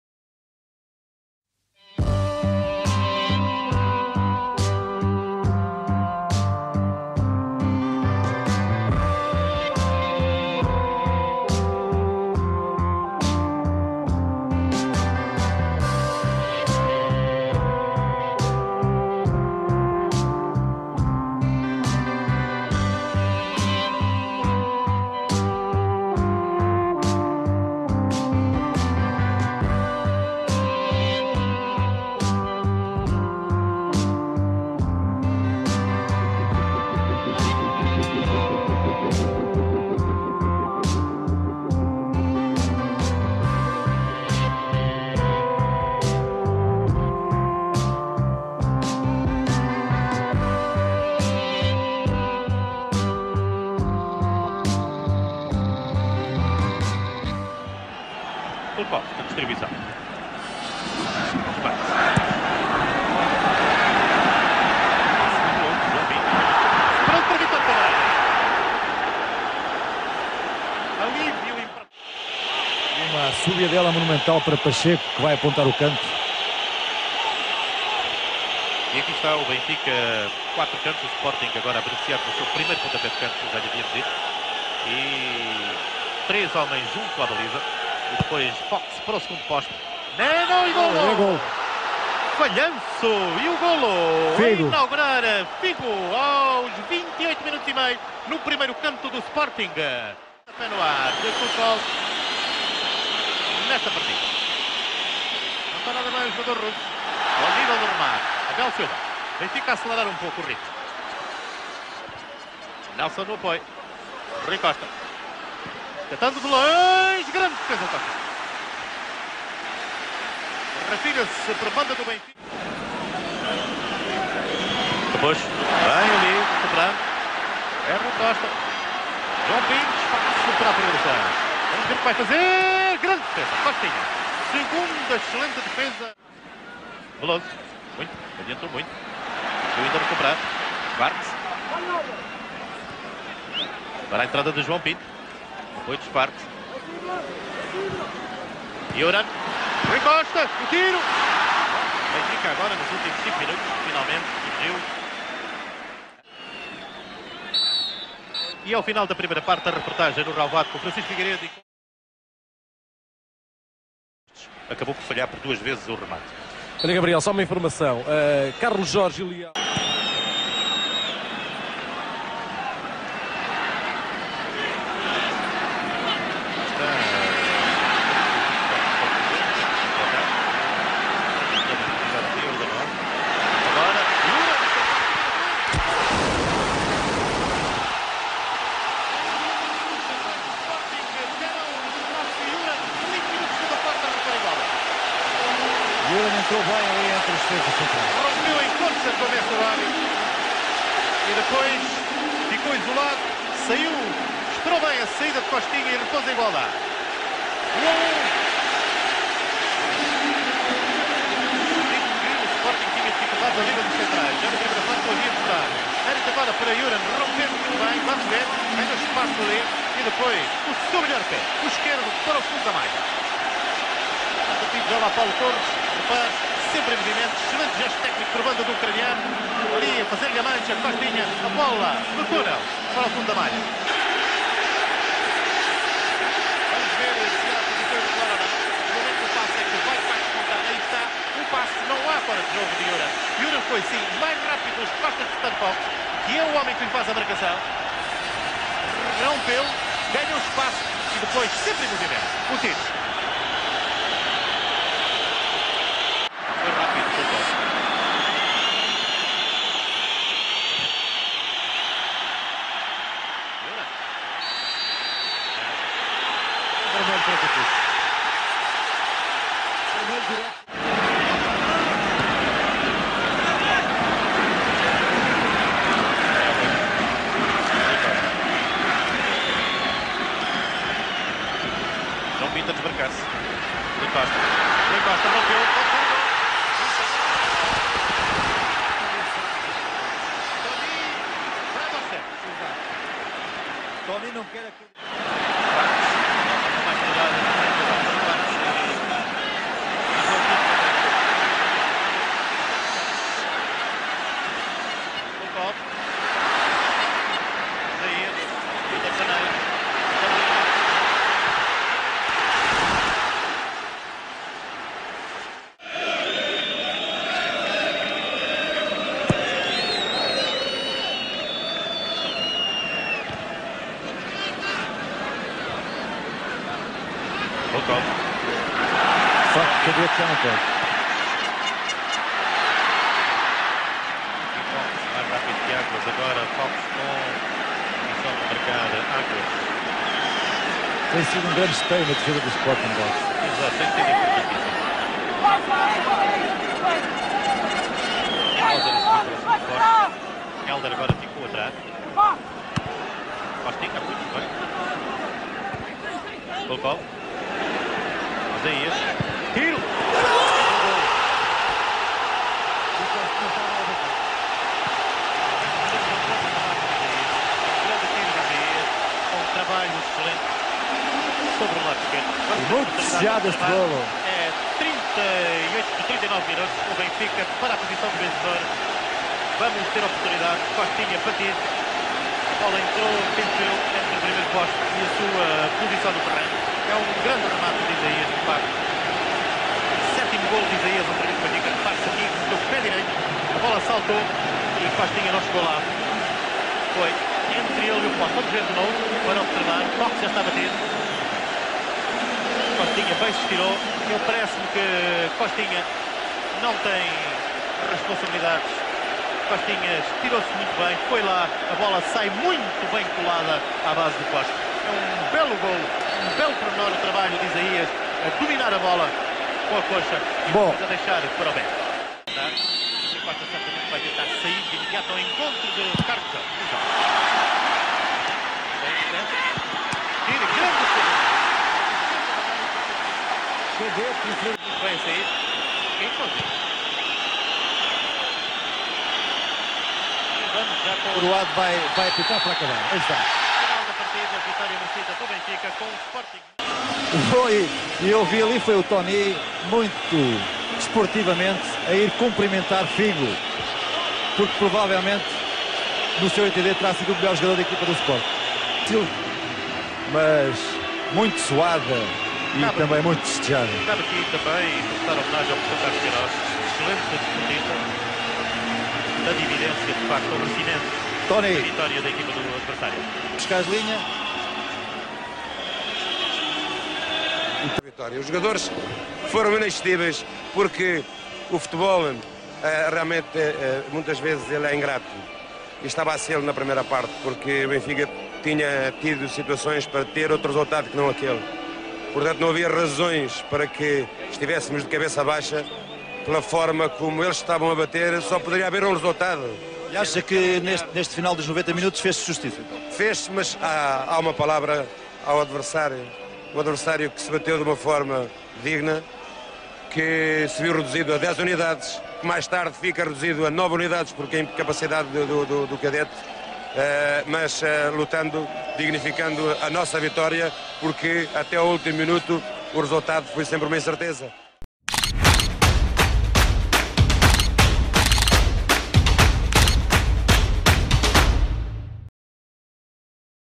Benfica 2 Sporting 1 (Época 1993 1994)
Partida referente à 13ª jornada do Campeonato de 1993/1994.

O Benfica vivia um período de convulsão... Jorge de Brito estava a ser empurrado para fora do clube e existiam dificuldades de tesouraria mas e citando Toni "Embora mergulhados numa crise profunda, temos dignificado a profissão, não esquecendo as regras de disciplina que se estavam a tornar lassas".

O Sporting? Estava a chegar ao Natal, vulgo a derrapar.

Grande partida foi esta entre duas grandes equipas. Intensa ao nível das disputas físicas. Com Toni a tirar um defesa ao intervalo e...

Este vídeo, acompanha o correspondente artigo no blogue: (Brevemente).

Subscrevam este canal, ajudem-no a manter-se vivo. Muito agradecido.